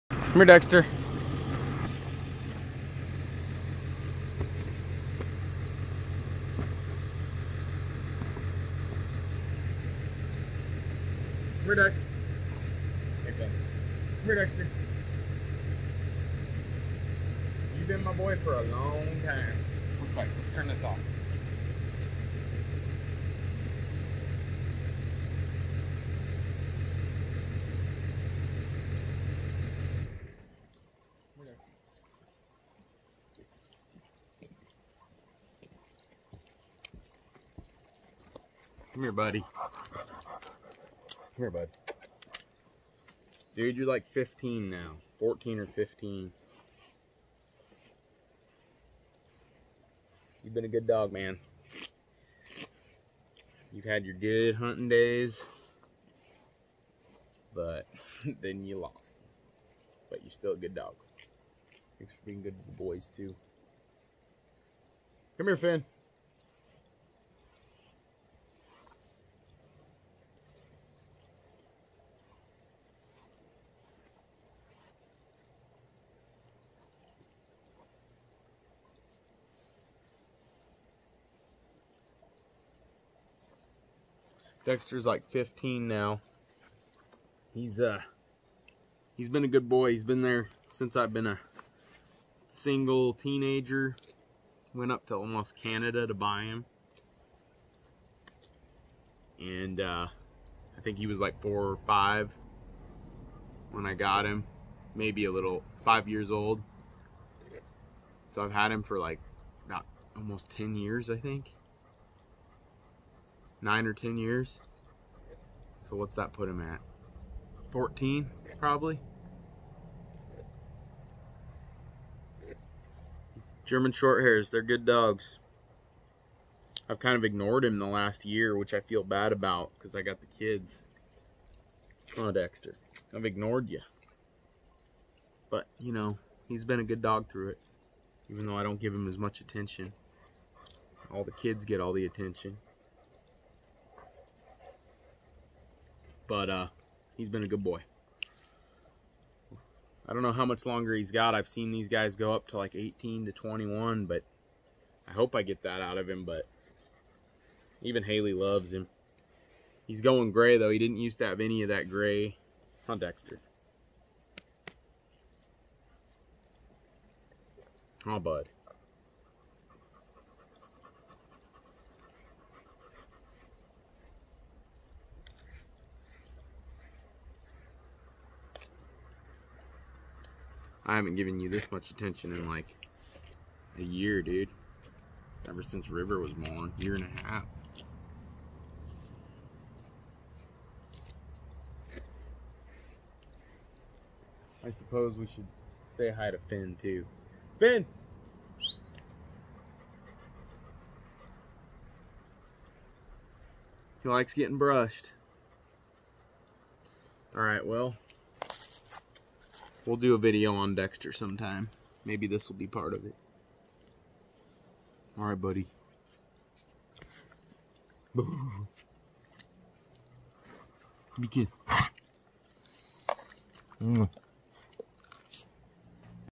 Dexter my 14 year old German Shorthair Pheasant Dog.
I've had this boy for a long time, he deserves an episode and this doesn't do it justice but figured you all might like to meet Dexter.